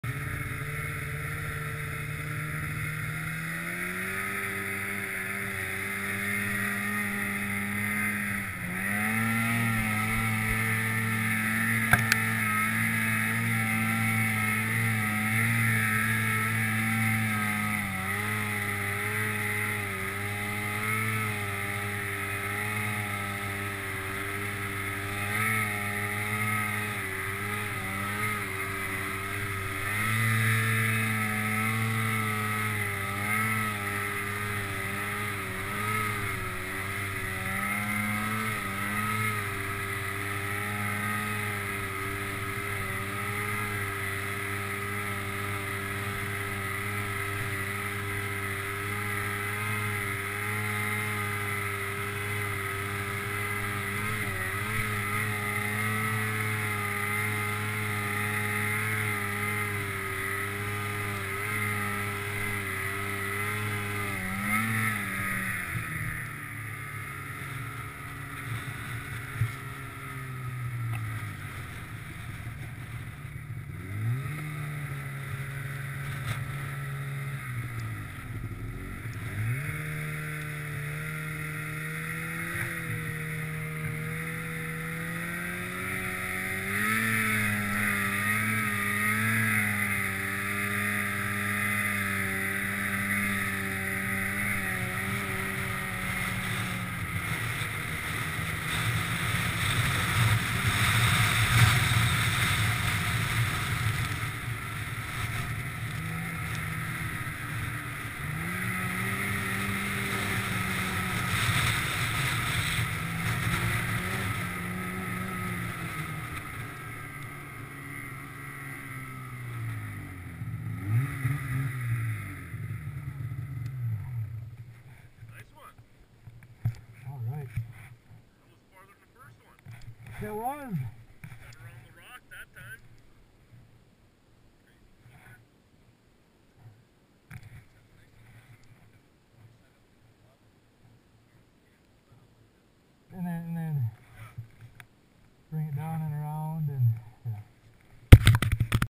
Pro Rmk Side hill
Side hilling a fairly hard pack hill in the Big Horns